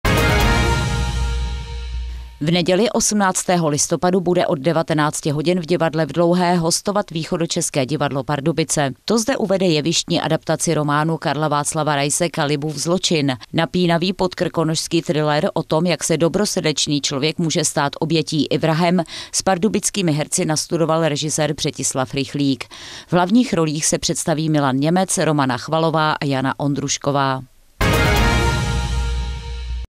Kalibův zločin, Východočeské divadlo Pardubice - hostování v Divadle v Dlouhé, 2018
pozvánka PRAHATV na představení v Divadle v Dlouhé